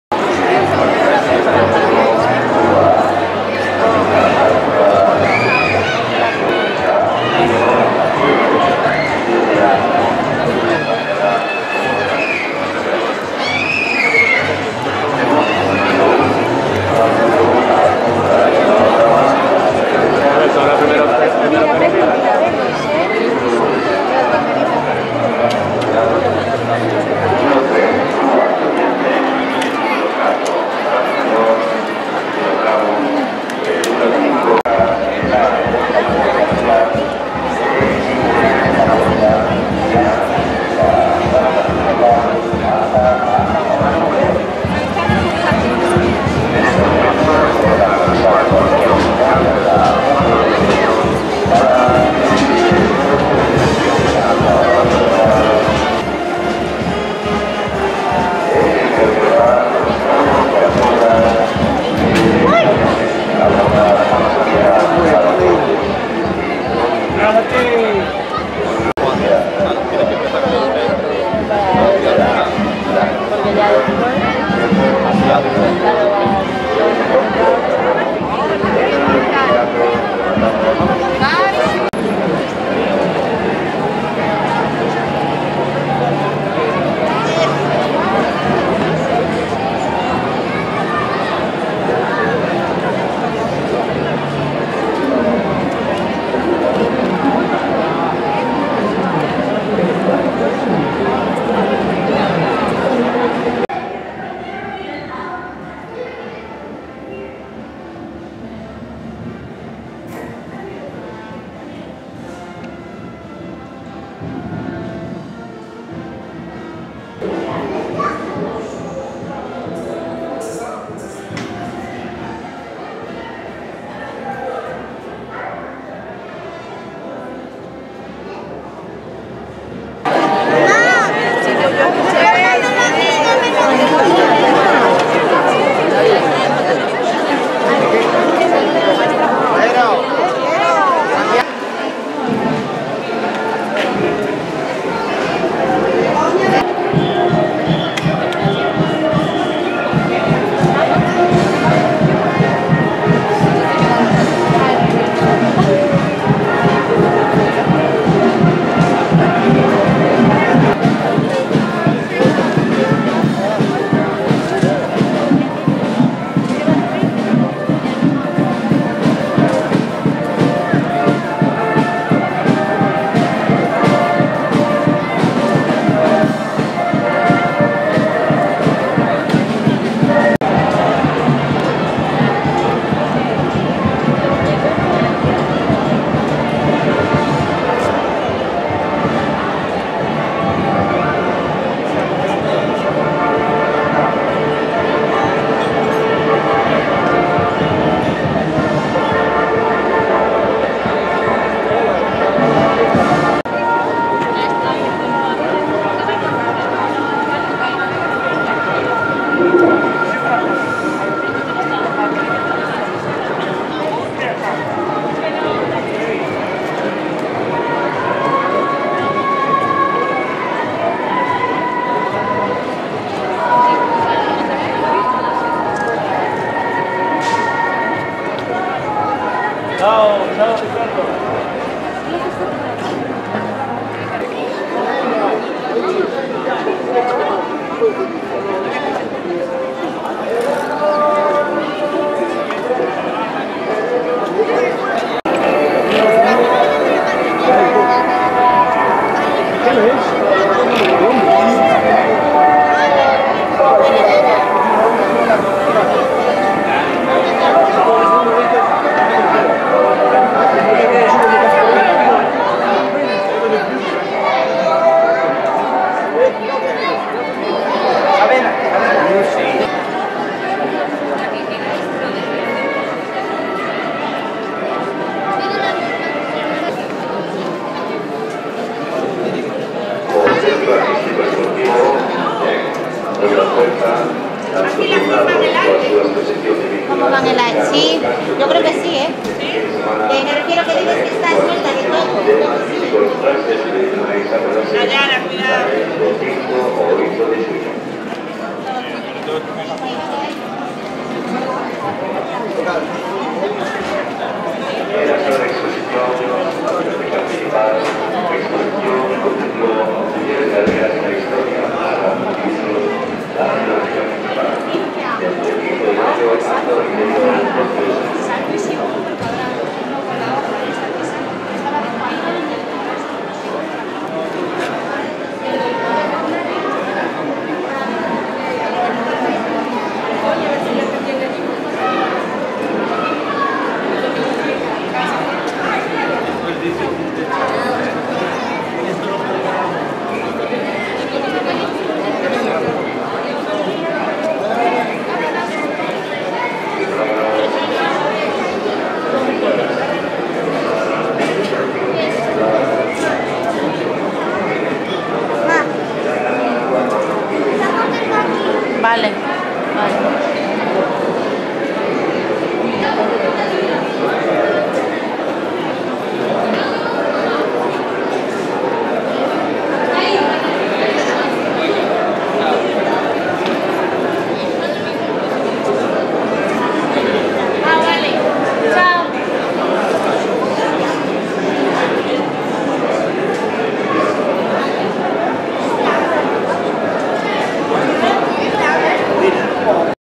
Corpus Christi Puenteareas 2015
Alfombras de flores para la fiesta del Corpus Christi de Puenteareas.